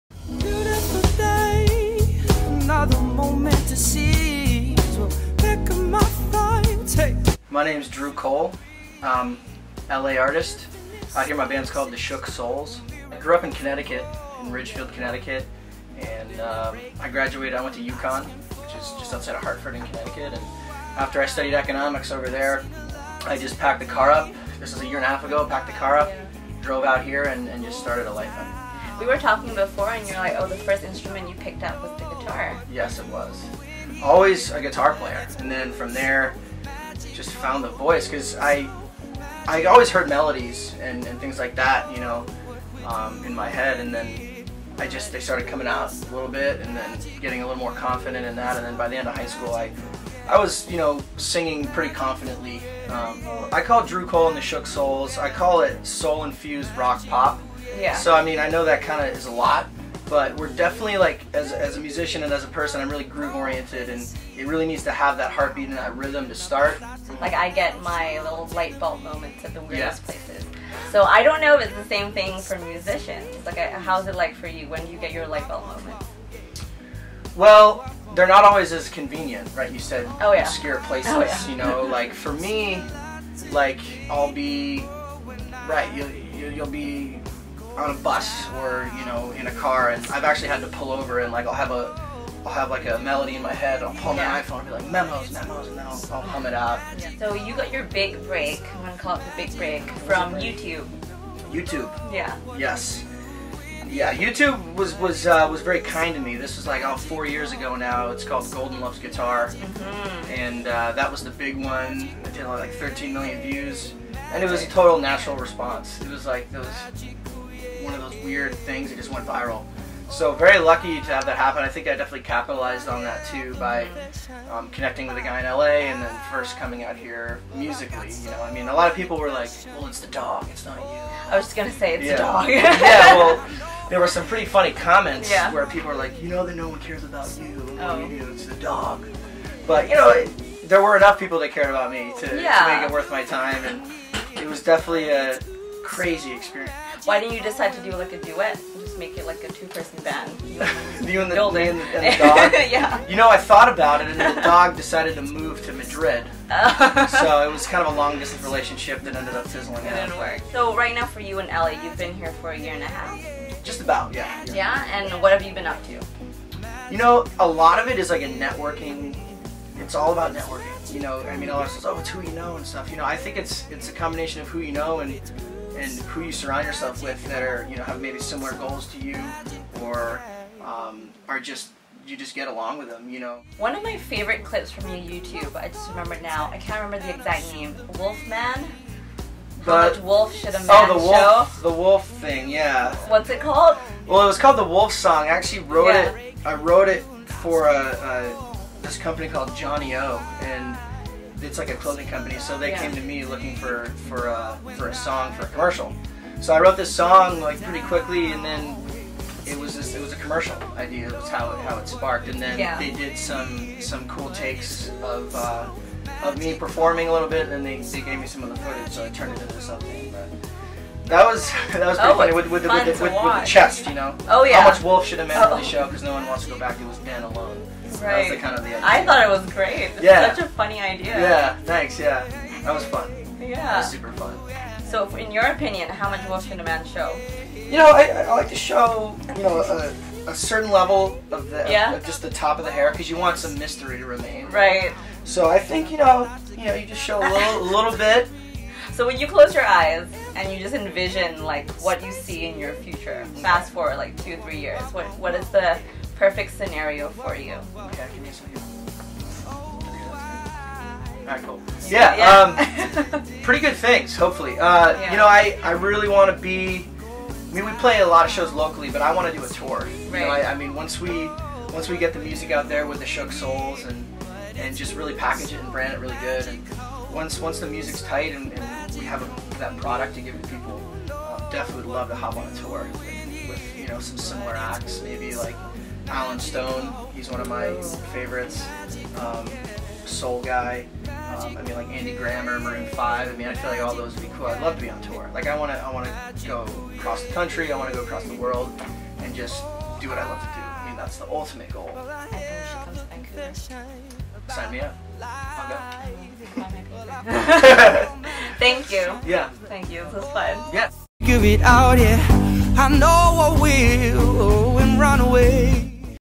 Sit Down with Drew Cole
During my visit to Los Angeles, I caught up with LA Singer-Songwriter Drew Cole (of The Shook Souls)!

Join me as we talk music, the blonde who shot his Youtube career to fame and just how much wolf should a man show ;)

Instagram.com/SamanthaSito
Facebook.com/otisamantha
Twitter.com/otisamantha
Pinterest.com/SamanthaSito